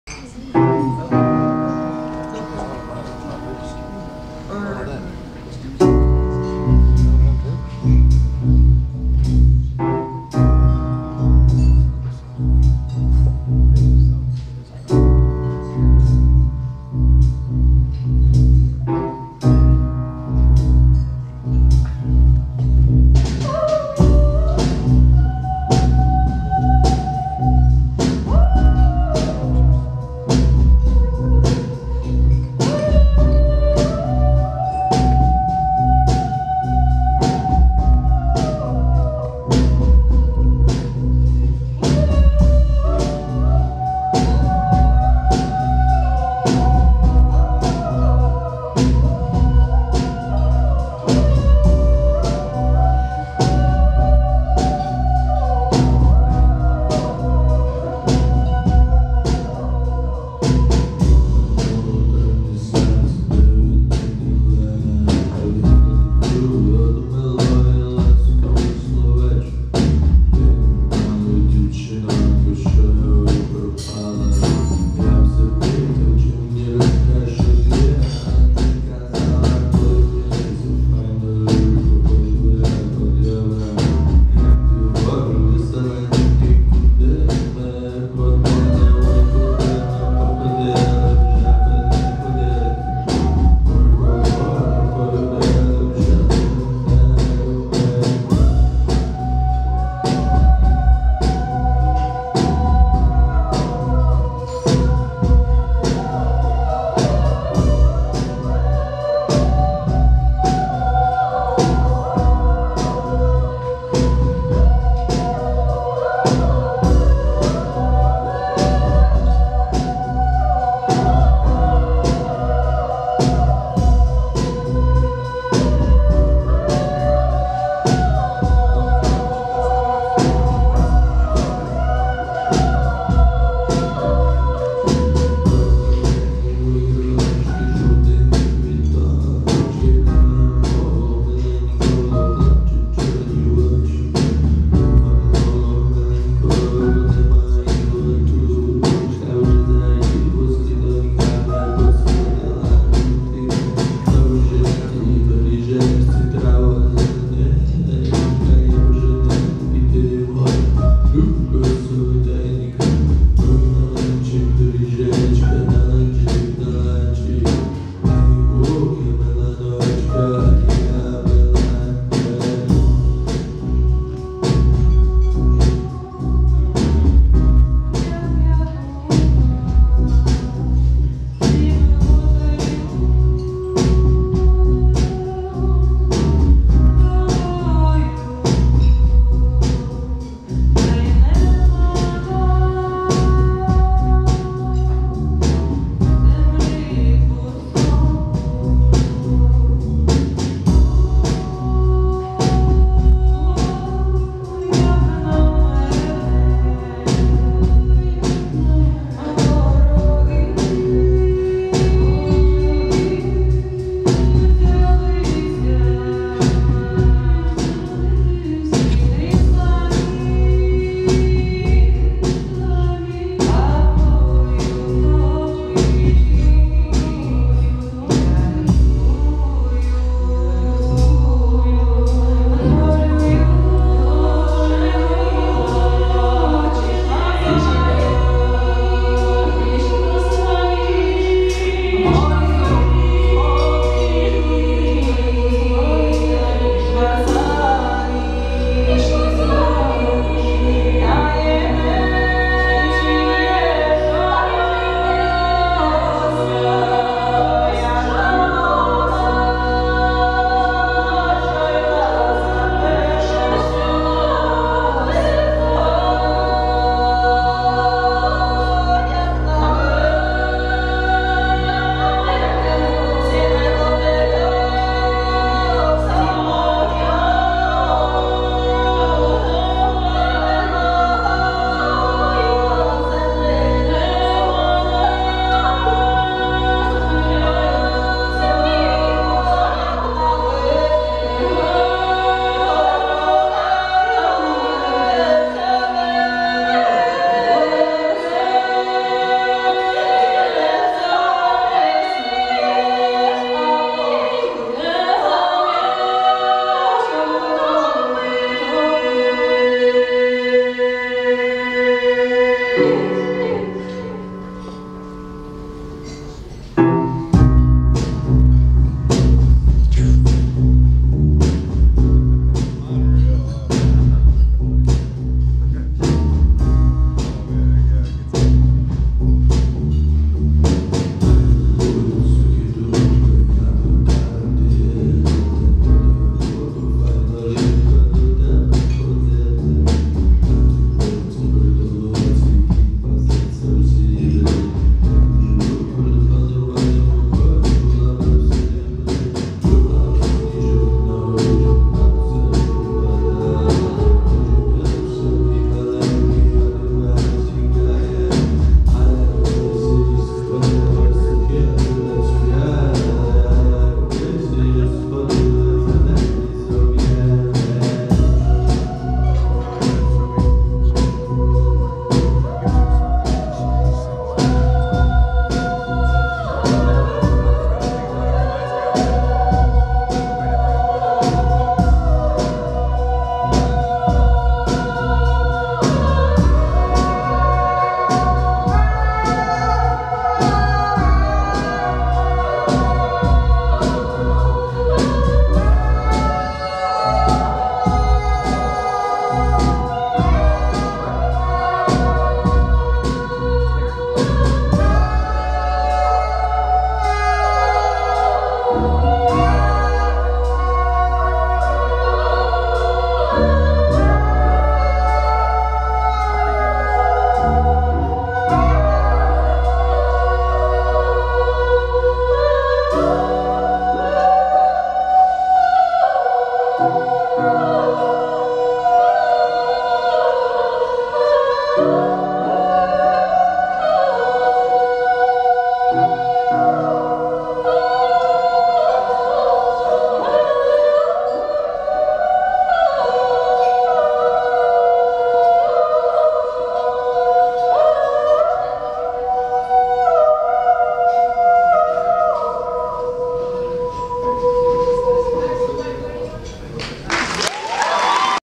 Dakhabrakha live at the City Winery in NYC 4/6/22 - Дахабраха - Нова пісня
New Song by Dakhabrakha live at the City Winery in NYC 4/6/22